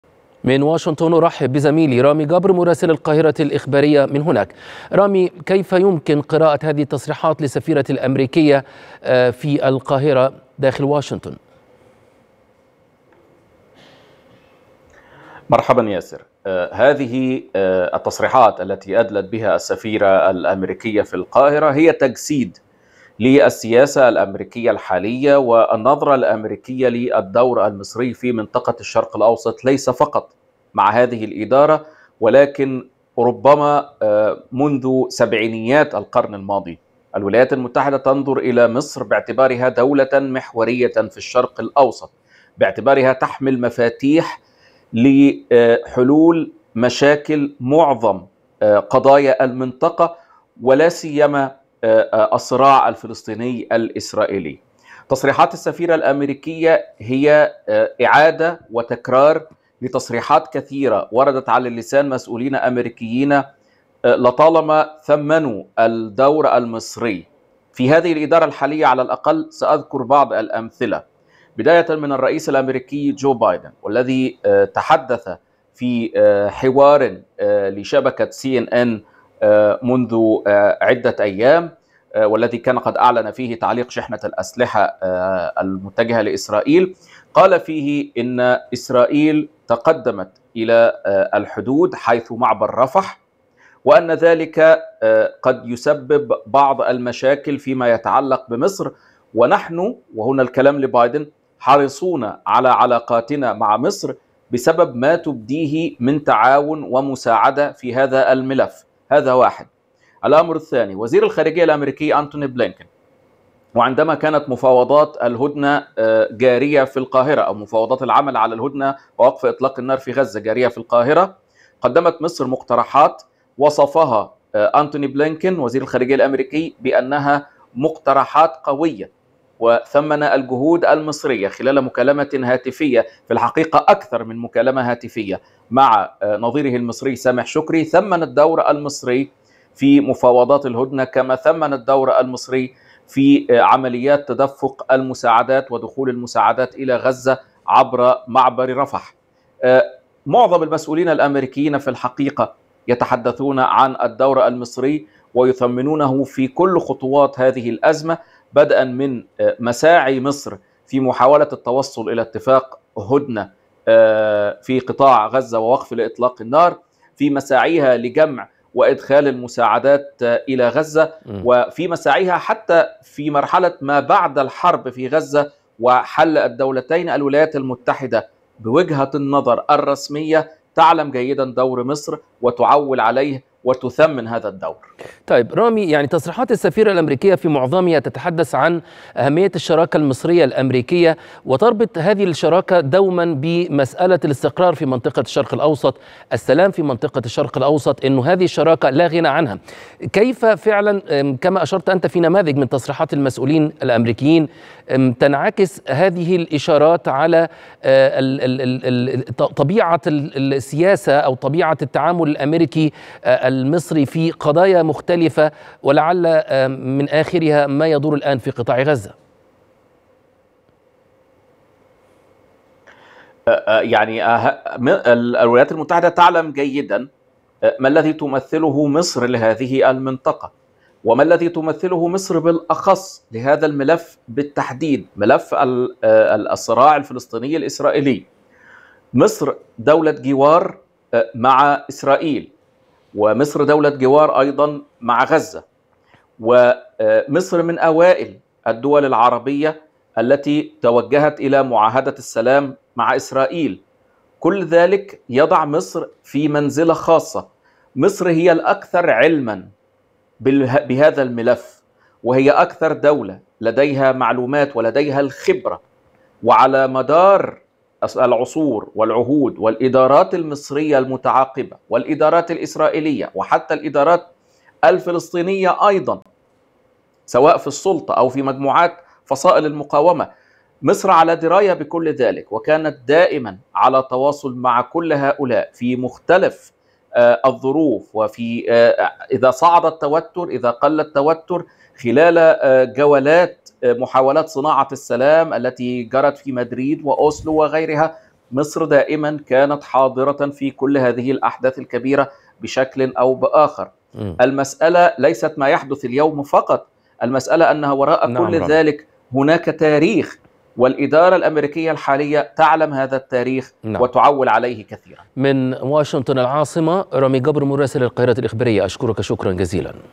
مراسلنا: الولايات المتحدة تنظر إلى مصر باعتبارها دولة محورية في الشرق الأوسط
مراسلنا من واشنطن رامي جبر: الولايات المتحدة تنظر إلى مصر باعتبارها دولة محورية في الشرق الأوسط

23/5/2024